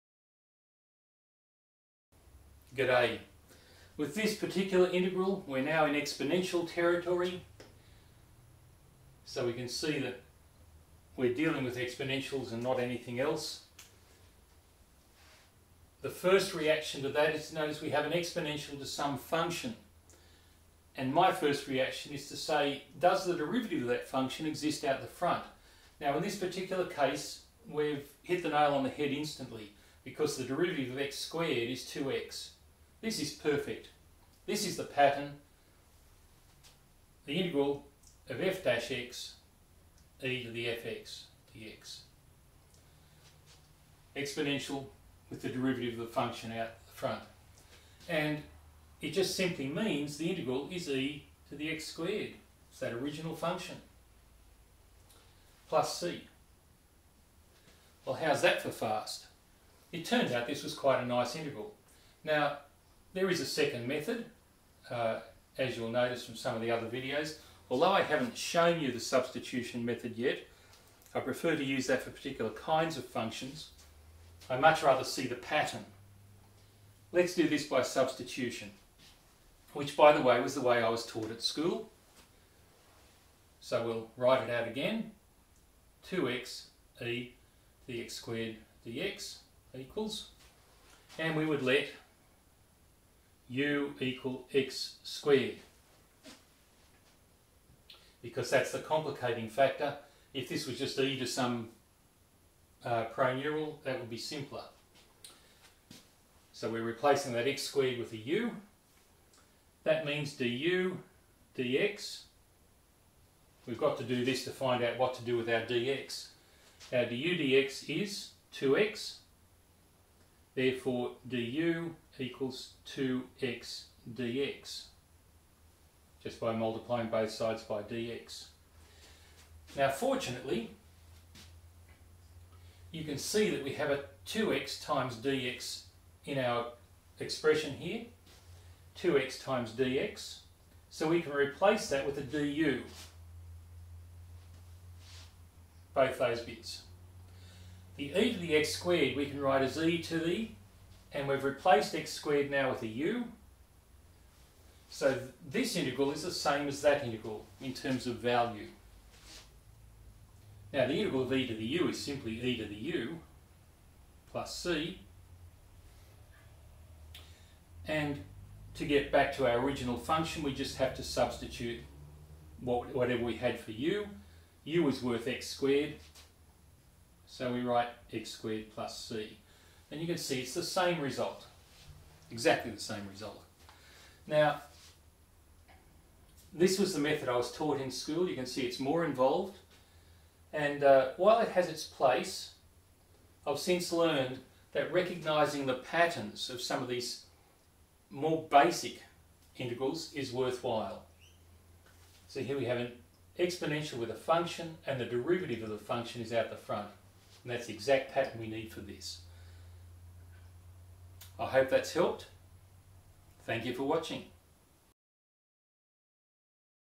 Basic Integration Example 06 (To Substitute or Not ~ Exponential)
This is the sixth of 21 videos, each of which is devoted to solving a 'basic' integral problem.  I have assumed that you have watched the previous videos in this series (and practised the skills discussed therein), or that you have learned integration at school and are moderately proficient with the basic techniques.

My aim is to discuss strategies and patterns ... things to look for and traps that you may encounter.  In other words, I try to emulate the kinds of thoughts that may go through a student's mind when confronted with different kinds of integrals.

I will not discuss each integral in the text here, so the text will remain essentially the same for all 21 videos (sorry ... you will just have to watch the videos ^^).